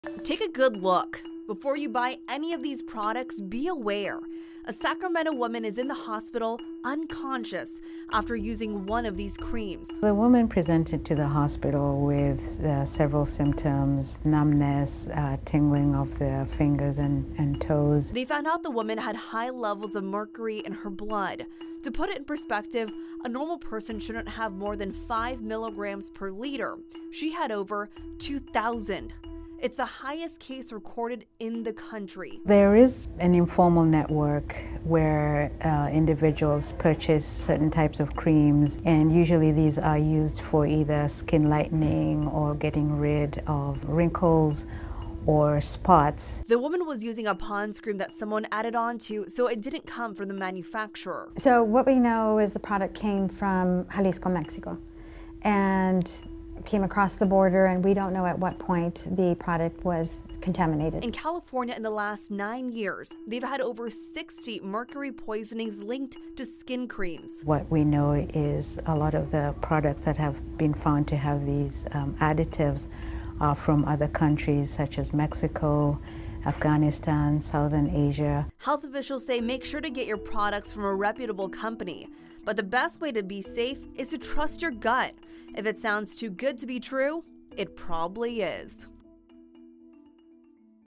Sacramento woman in semi-comatose state after using face cream tainted with mercury
A Sacramento woman is in a semi-comatose state after using a Pond's cream that was tainted with methylmercury. The cream was purchased from Mexico, but it is unclear where the mercury was added.